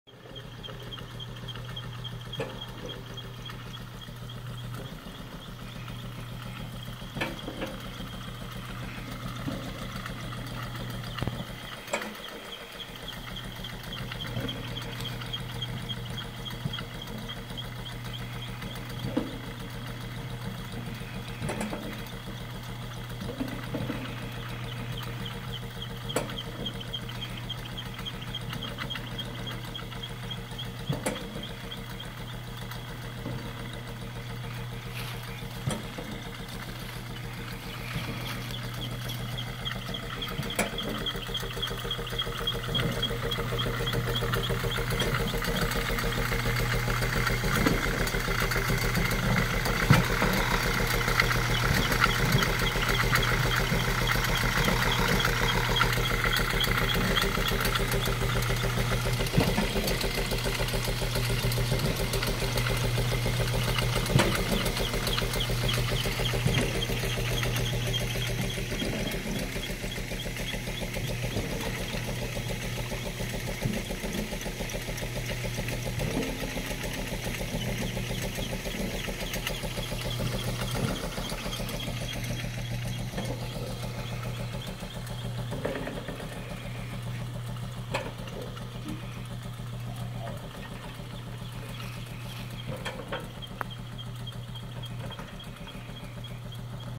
Pumpjack in the Allegan SGA
Apparently there's still oil under Michigan.